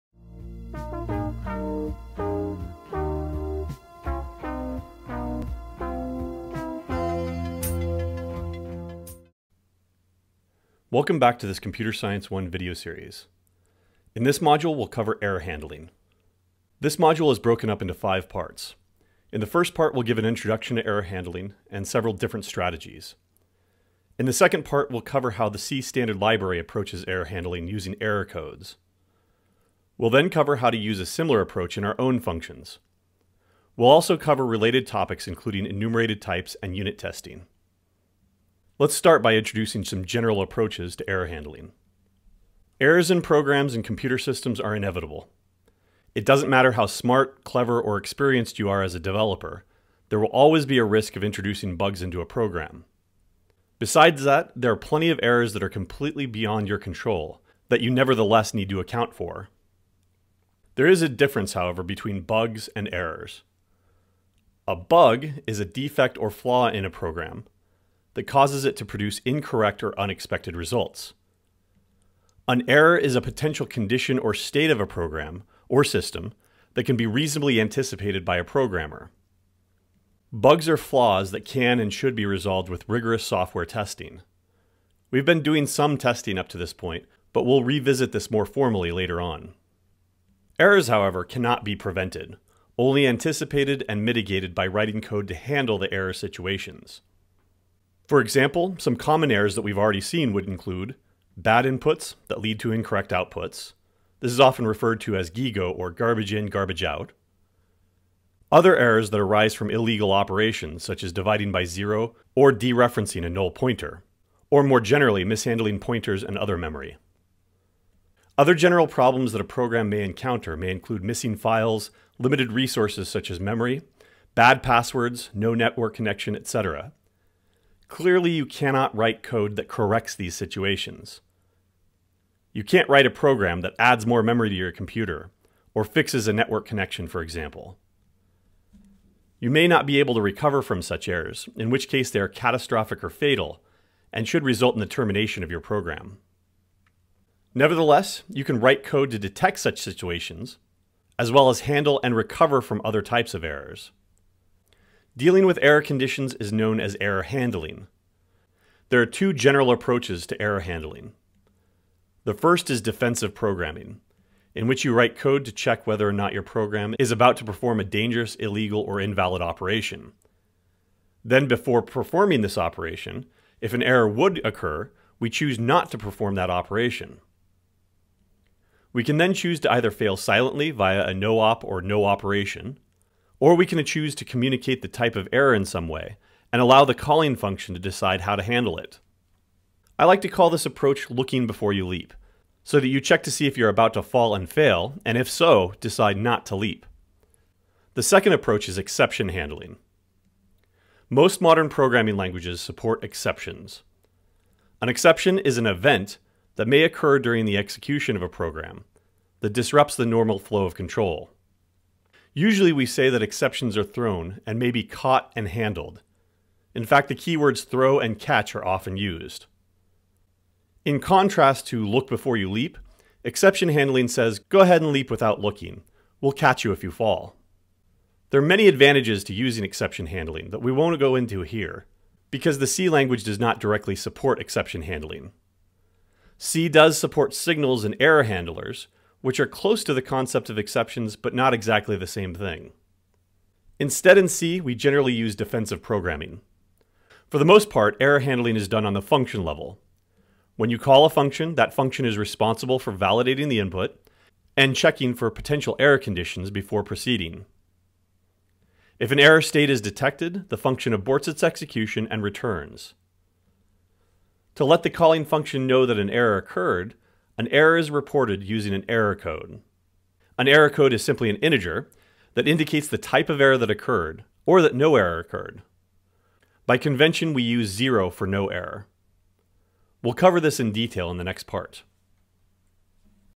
6.1 - Error Handling - Introduction
An introduction to error handling strategies.

0:00 Error Handling
0:38 Introduction
2:35 Error Handling
3:20 Exception Handling
4:04 Error Handling in C